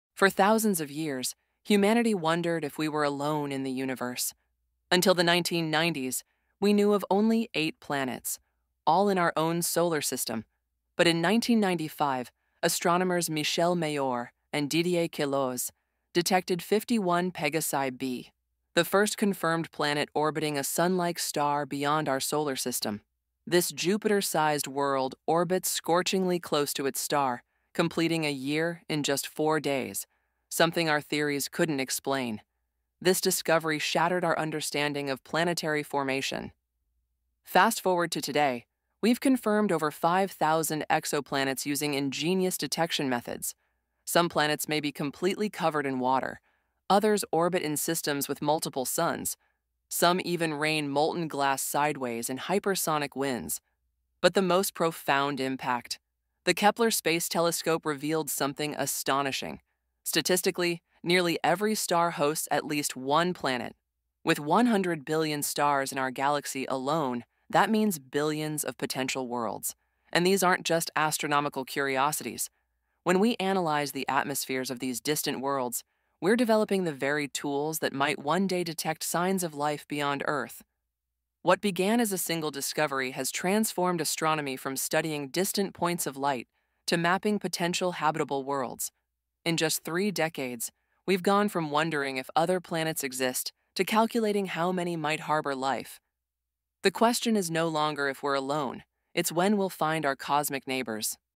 Why Exoplanets Revolutionized Astronomy Forever
Scientists thought our solar system's eight planets might be all there was until the groundbreaking discovery of 51 Pegasi b in 1995 changed everything. Now with over 5,000 confirmed exoplanets, astronomers estimate our galaxy contains billions of worlds, many possibly habitable.